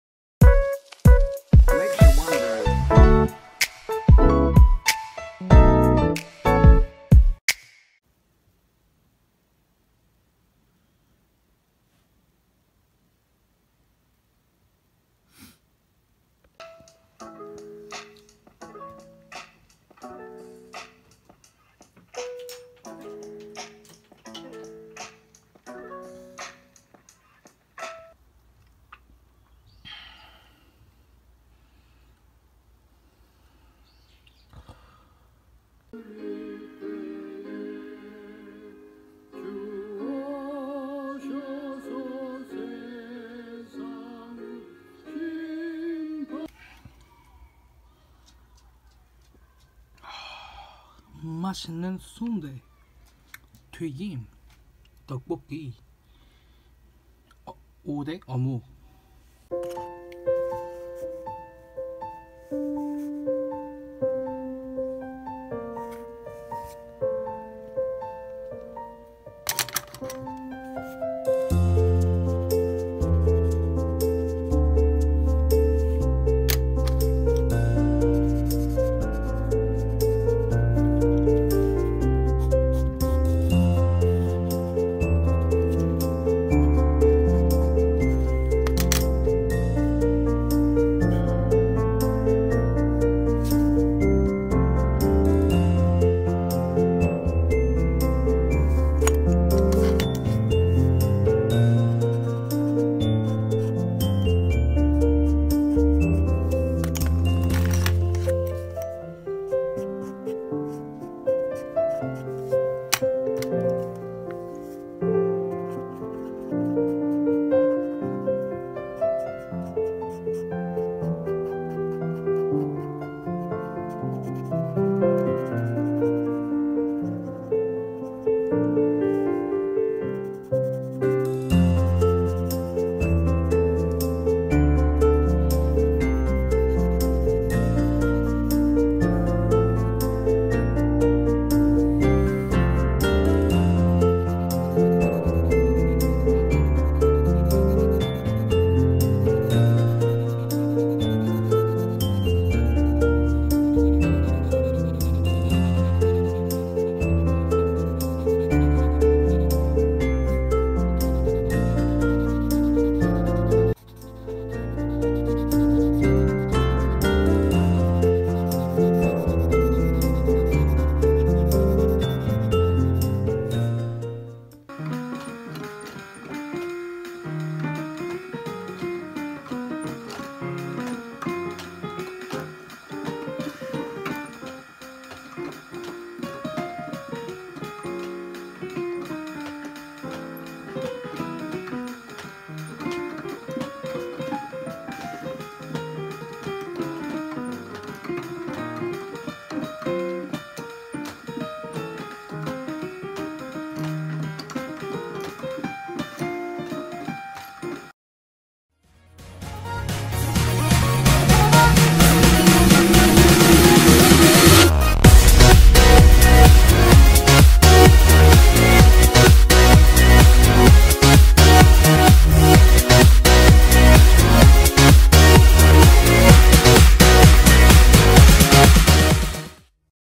🇰🇷 EP. 194 한국 이야기 | 제주 브이로그 | 자가격리 D-8 | 네덜란드에서 와서 자가격리 해야하는 음대생의 주말.. 온라인예배, 피아노치기, 그림그리기, 그외다양한것
vlog

자가격리 8일 남았다.. 미쳐버리겠다..
밖에 나가고싶다.. 항상 온도 체크히고..
온라인으로 예배 드리고.. 비트 만들고.. 그림그리고..
연습하고.. 밖에 나가서 오랜만에 친구랑 소주한잔 하고 싶다..

Instsgram : 99.blank